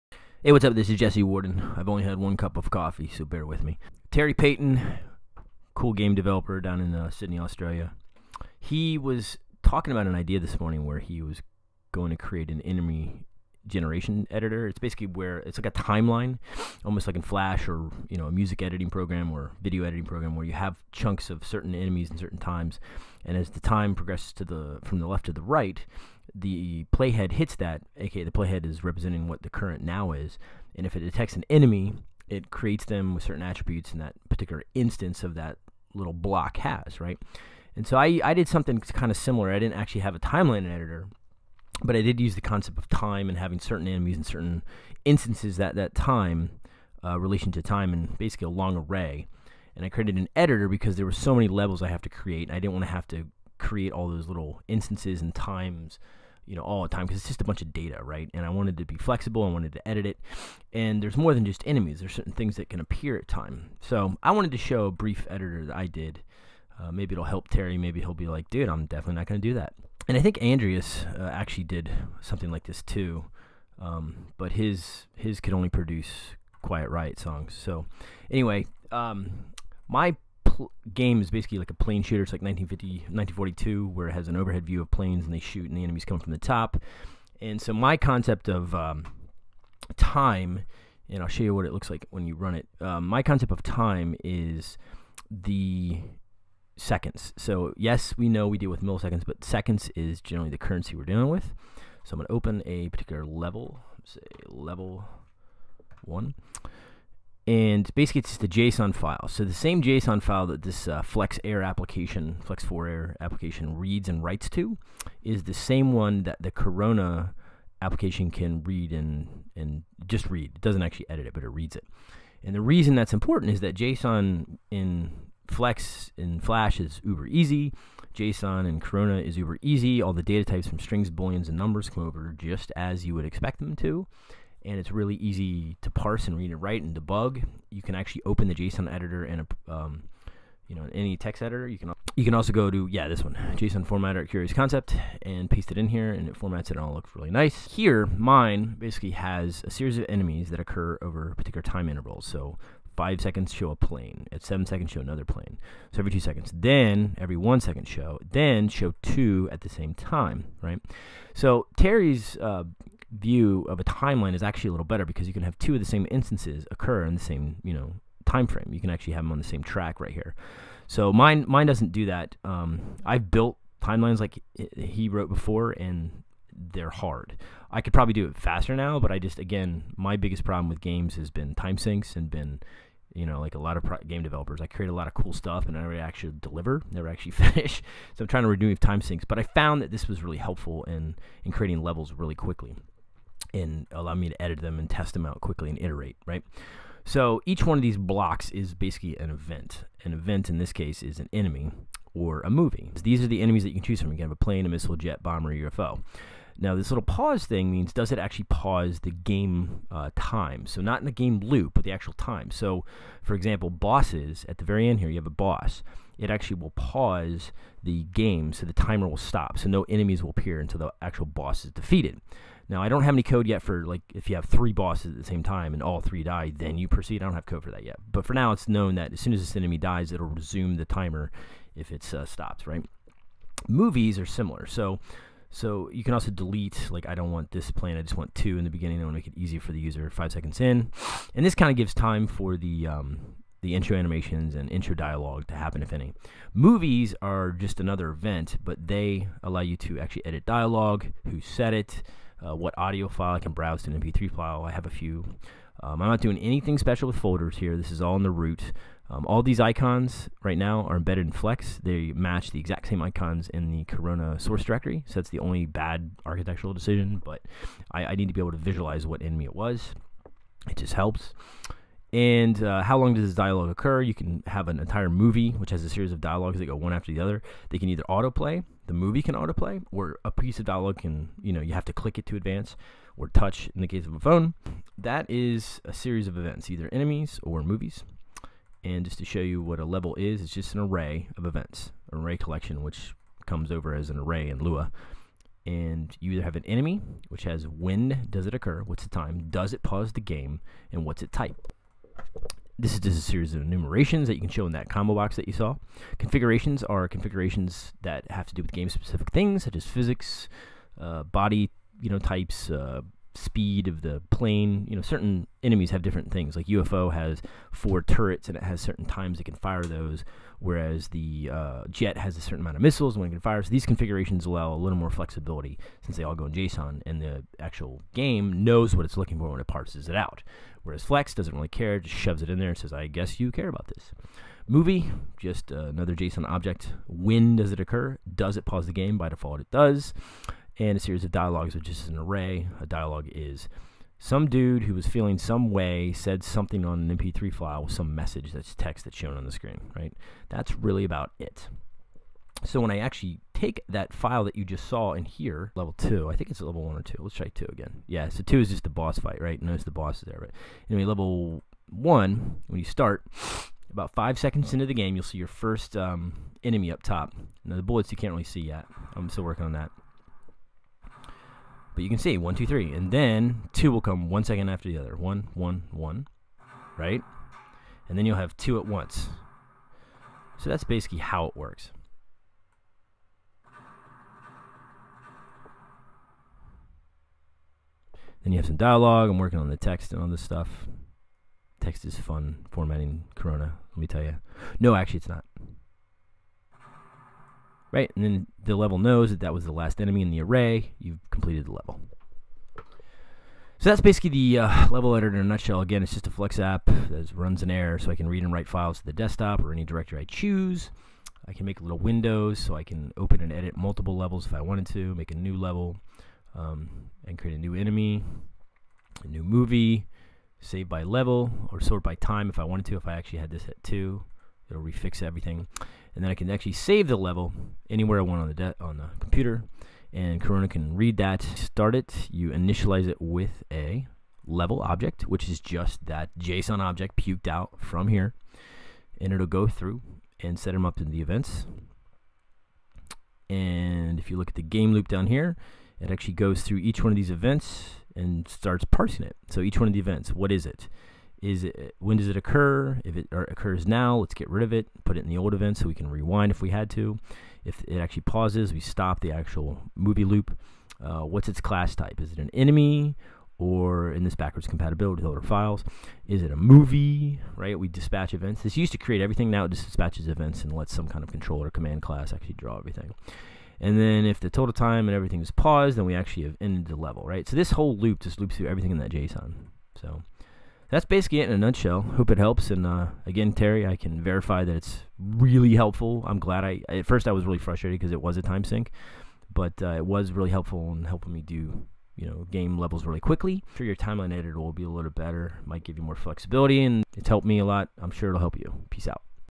Level Editor - Enemies in Time
I show a level editor built in Flex 4 AIR that reads & writes a JSON level file that a game in Corona SDK can use to show enemies and movies at specific time intervals. Hoping it shows +Terry Paton the effort is worth it.